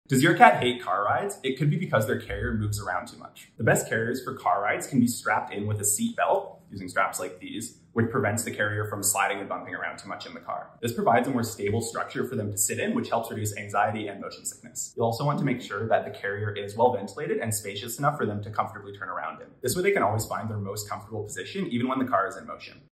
Does your cat HATE CAR RIDES?!
Learn about the common causes of carrier anxiety and how to recognize the signs. Create a "home away from home" for your pet inside their carrier.